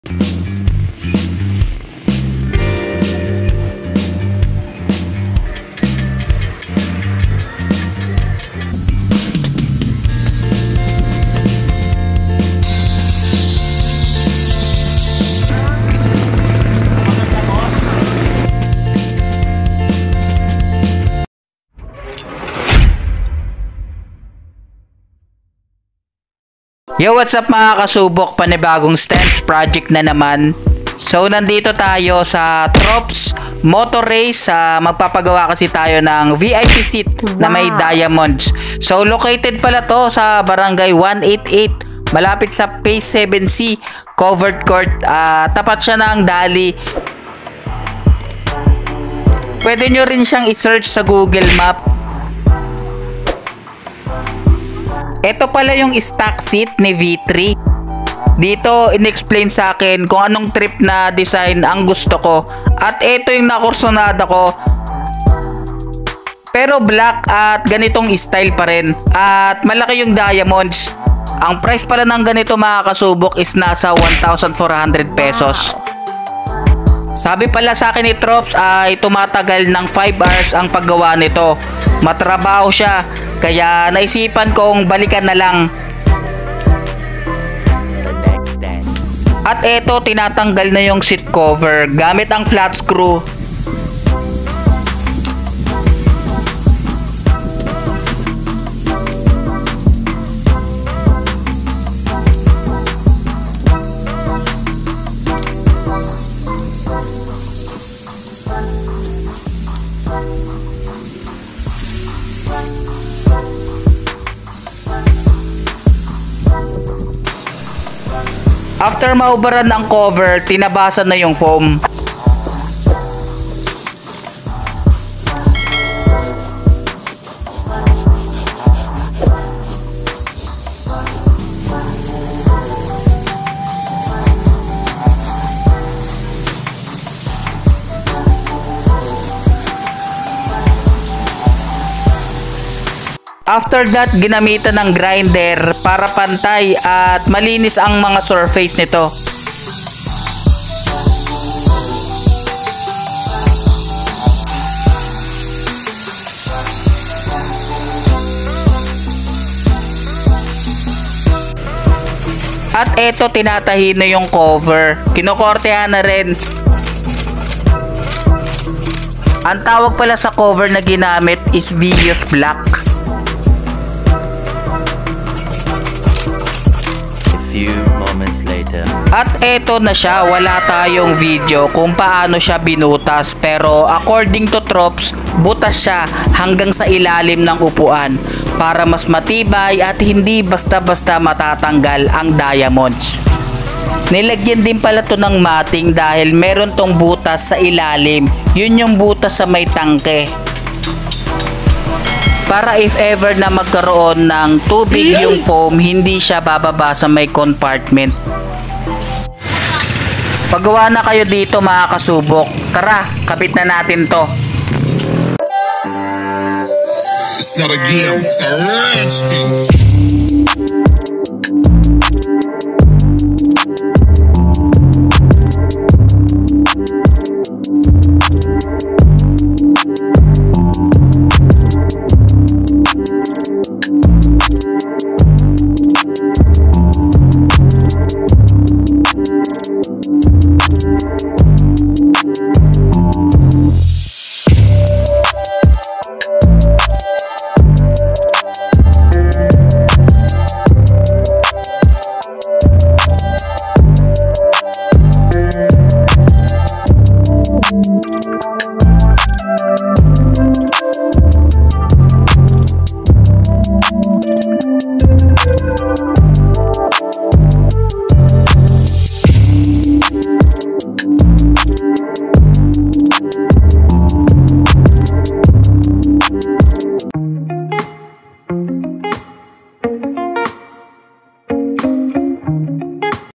VIP SEAT W/ DIAMONDS - EASYRIDE150CL - STANCE PROJECT EP.7
Nagpagawa pala Ako Ng vip seat at talagang nagustuhan ko Ang resulta, kaya panoorin mo kung pano Siya ginagawa!

FIFTH TV

FIFTH TV
FD
59X

TEAM KASUBOK FD